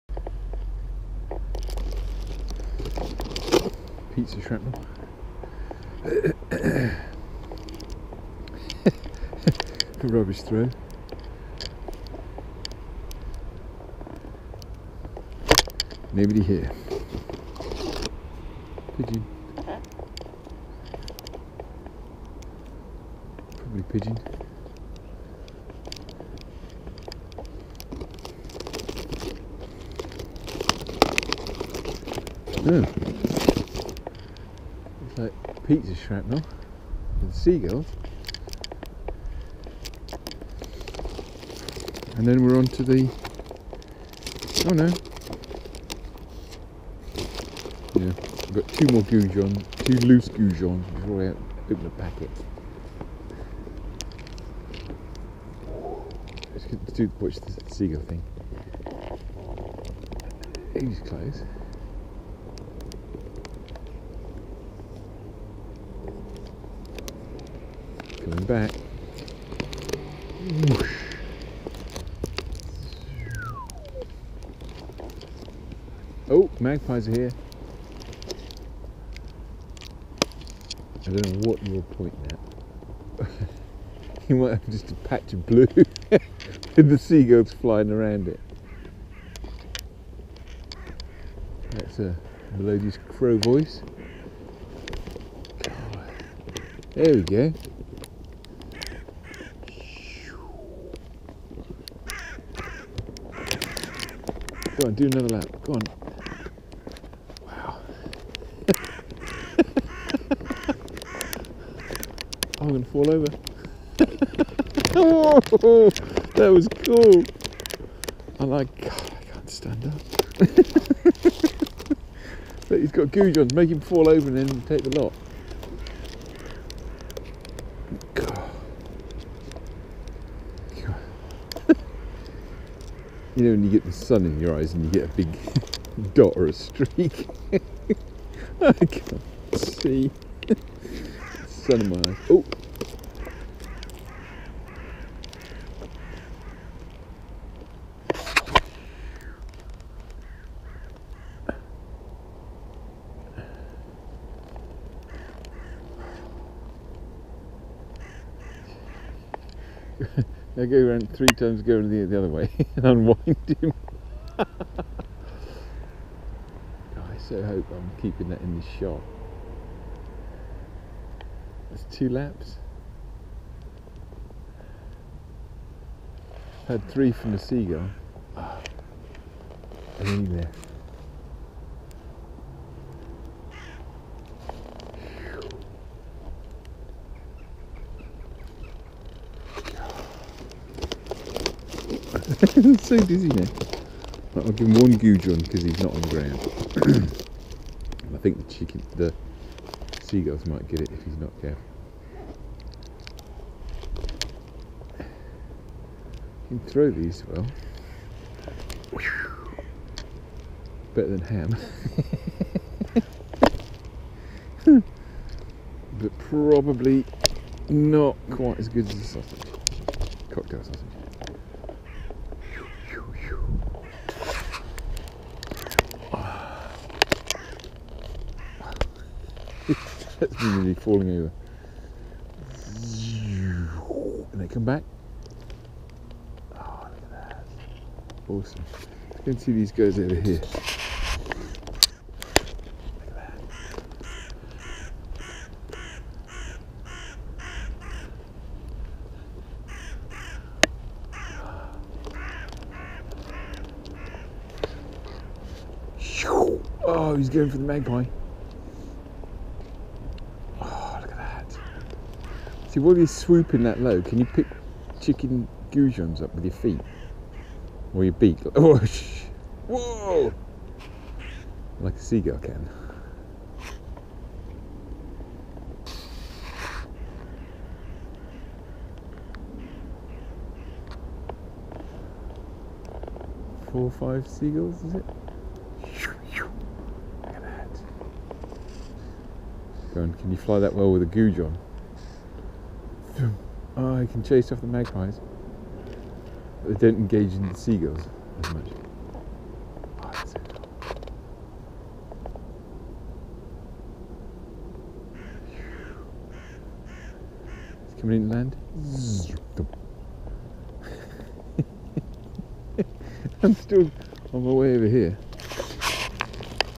White Table Woods. feeding crows. 1766 two loose goujons
This year Ive taken to feeding the crows in the park at dinnertime. Some of the clips are amusing, but you might need to watch two or three to find a funny one. You can see the birds grow up during the year. Im learing about bird territories.  Birds can see in UV, what do crows look like to each other?